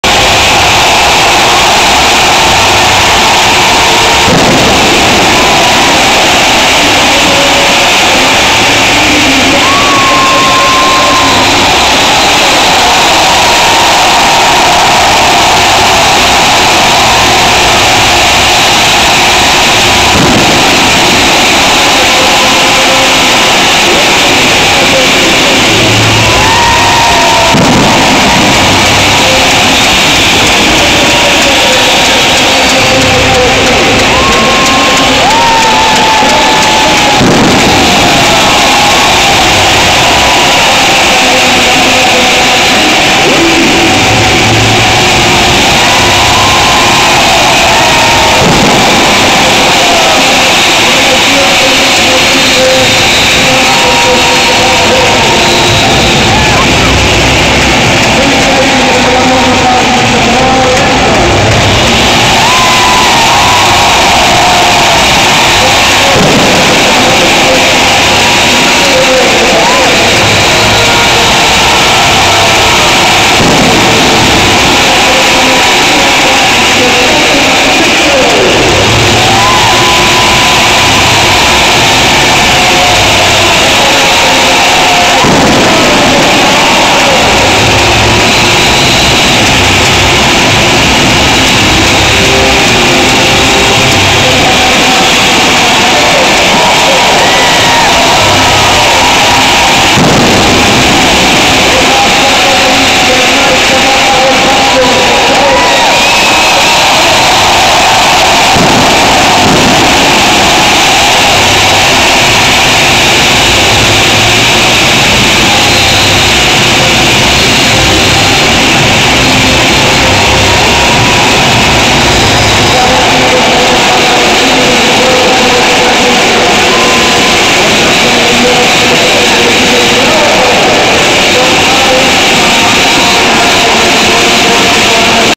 Last Game NFL Europe in Frankfurt Germany
Einlauf der Galaxy beim Worldbowl 2007 in Frankfurt